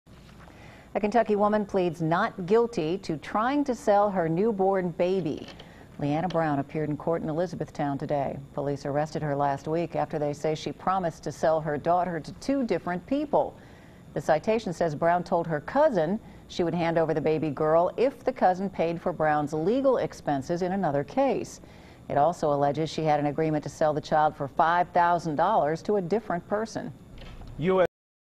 Woman Pleads Not Guilty of Trying to Sell Newborn Baby
HARDIN COUNTY, Ky. (WKRC) -- A Kentucky woman pleads not guilty to trying to sell her newborn baby girl.Lee-Anna Brown appeared in court in Elizabethtown Wednesday.  Police arrested her last week after they say she promised to sell her daughter to two different people. The citation says Brown told her cousin she would hand over the baby girl if the cousin paid for Brown's legal expenses in another case. It also alleges she had an agreement to sell the child for 5,000 dollars to a different person.